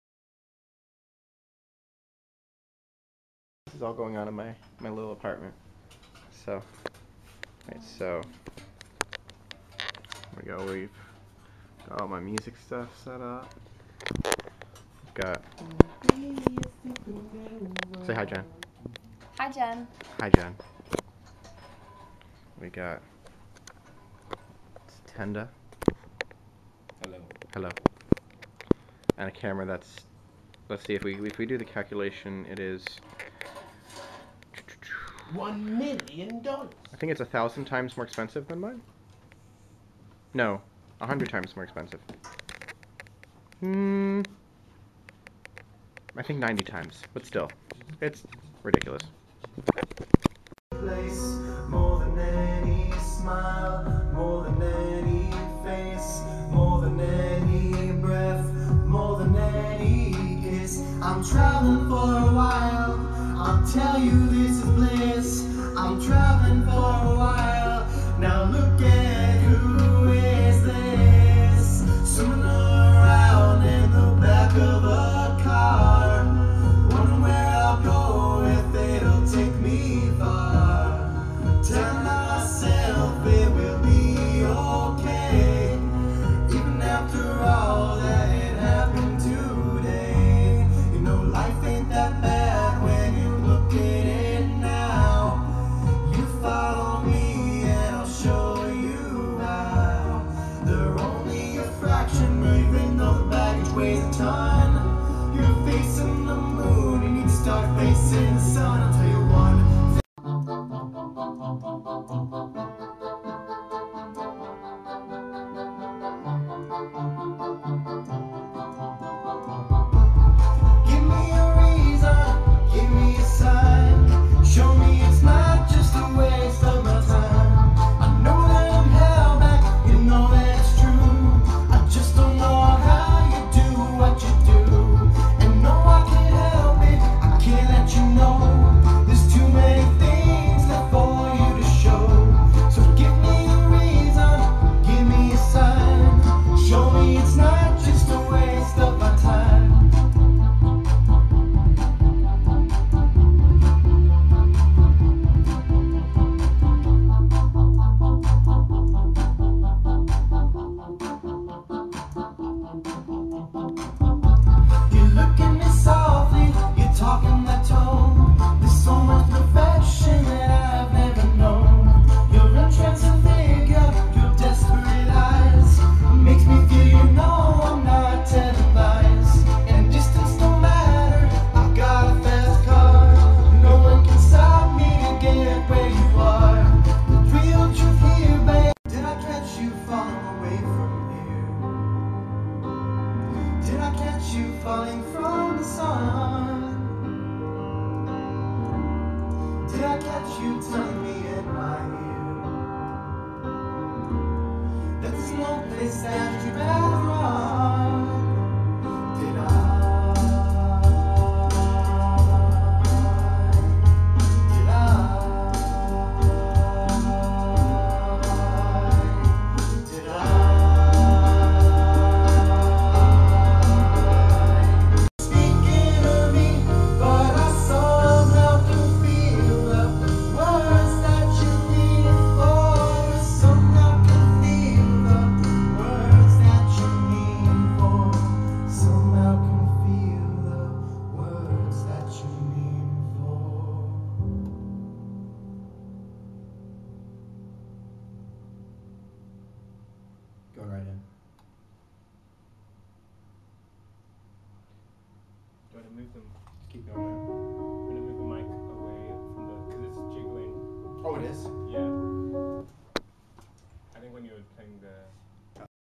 Behind the Scenes, Music Video
Behind the scenes at a recent performance/ interview video shoot at my apartment studio. In the video is Jen and tatenda, Filming with a gy-700.